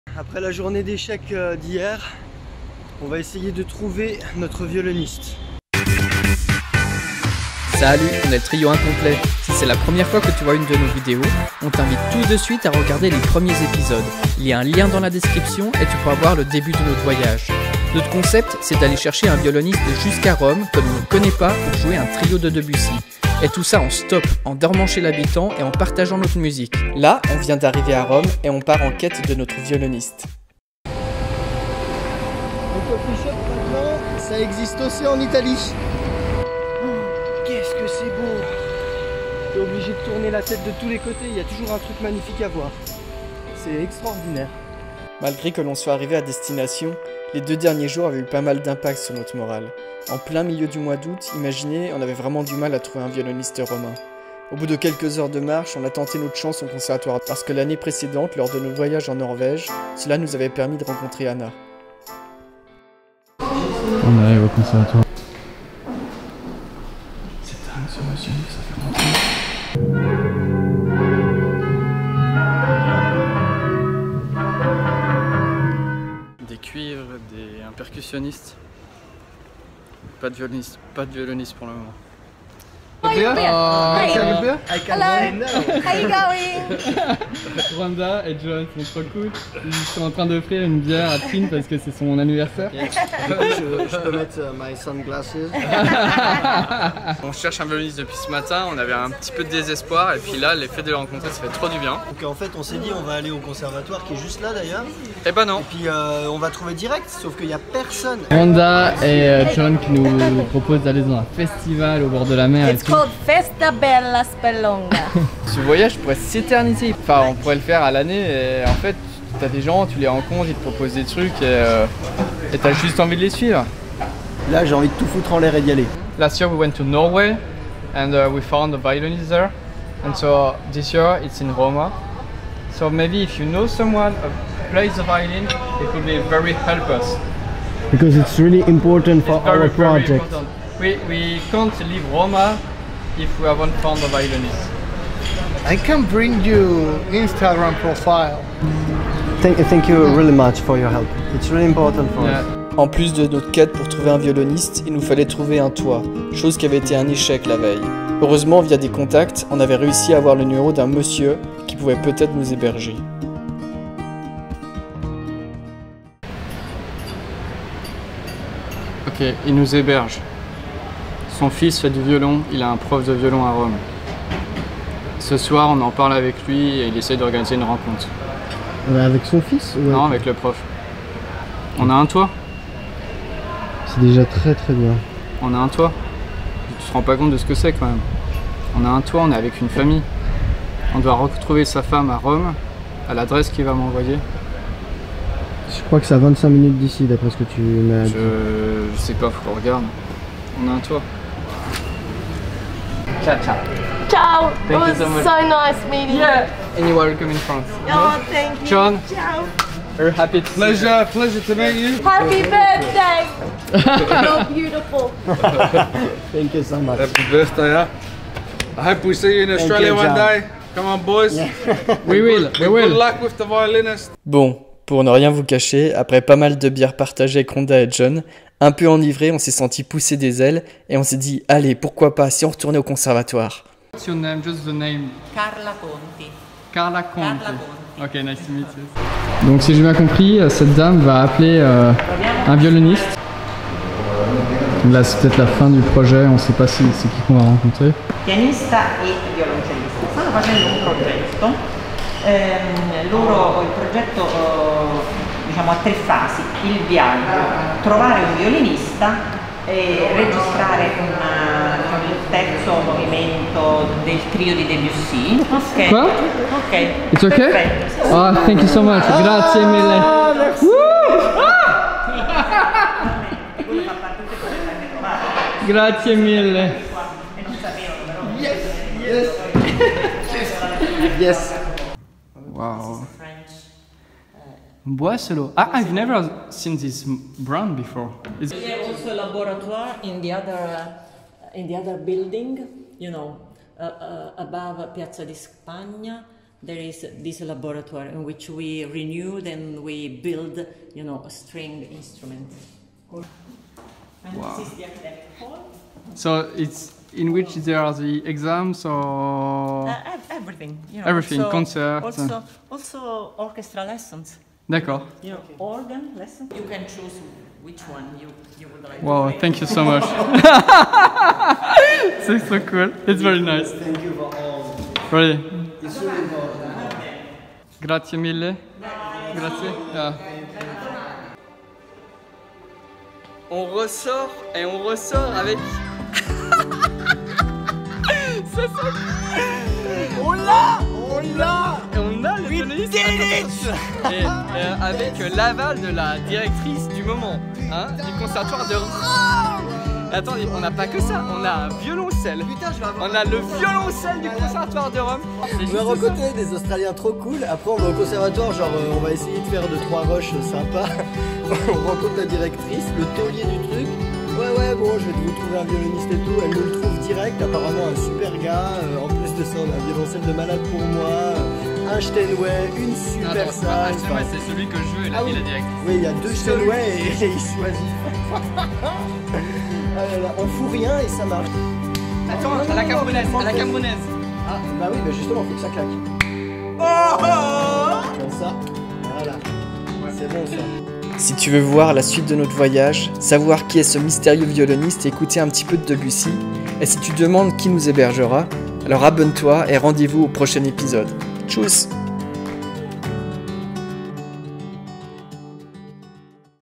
🇮🇹 ROME | EP10 ON Y EST PRESQUE (ENG SUB)
🇫🇷 Voici le 10ème épisode de notre série ! Nous sommes à Rome, en quête de notre violoniste.

🇬🇧 This is the 10th episode of our series "All roads lead to Rome". We are the Incomplete Trio. If it’s the first time you’re watching us, have a look at the first two episodes. We are currently in Roma, looking for a violonist. This new day promises to be full of adventures!

Traduction: Jennifer Peschier
Animation de la carte: Dylan Rey
Montage: Pierre Takahashi